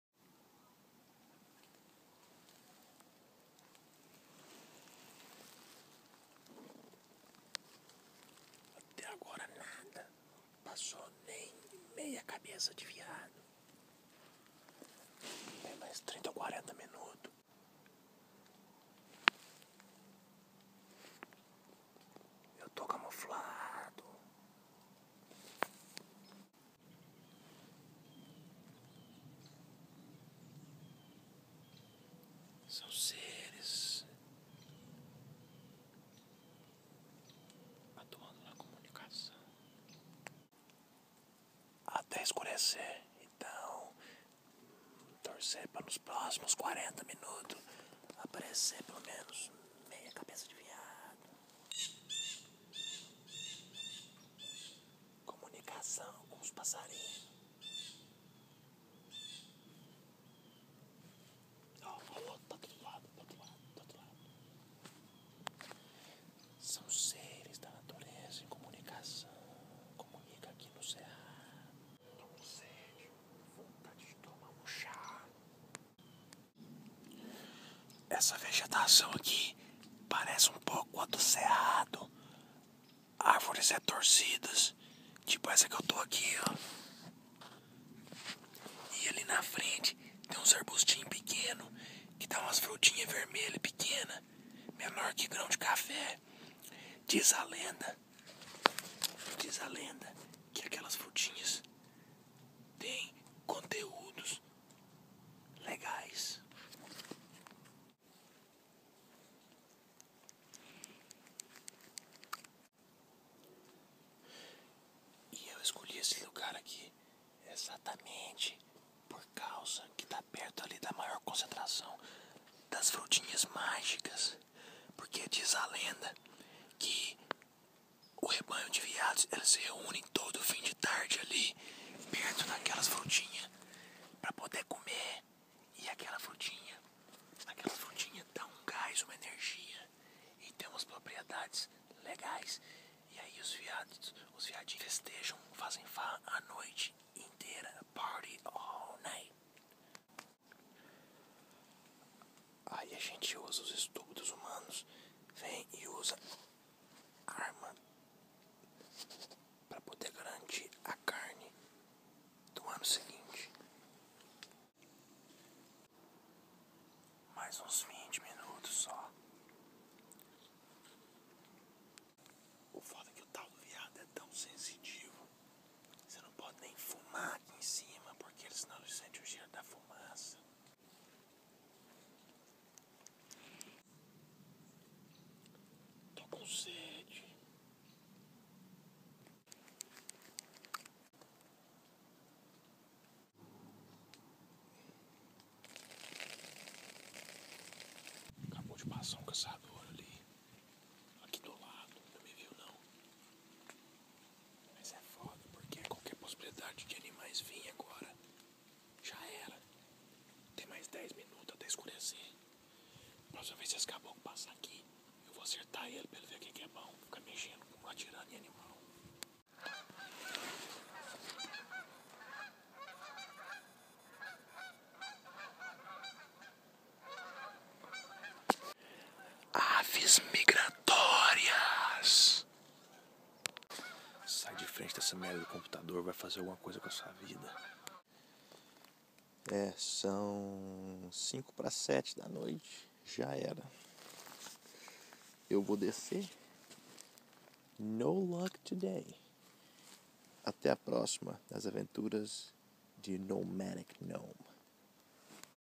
Cacada frustrada
relatos vindos de uma cabeca vazia durante um dia inteiro de espera numa cacada no cerrado no meio da tundra